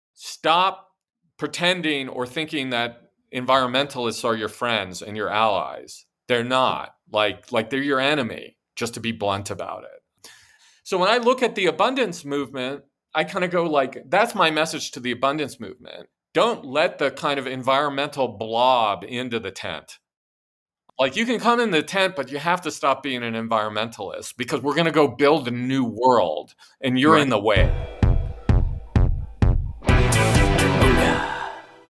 Stop being an enviromentalist: let's build a new world!
Ecomodernists had to realize, says Ted Nordhaus, that environmentalists aren’t potential allies; they’re actually standing in the way.

Ted Nordhaus, co-founder and executive director of the Breakthrough Institute, talks to Leigh and Alex about the 20th anniversary of "The Death of Environmentalism" and the 10th anniversary of "The Ecomodernist Manifesto."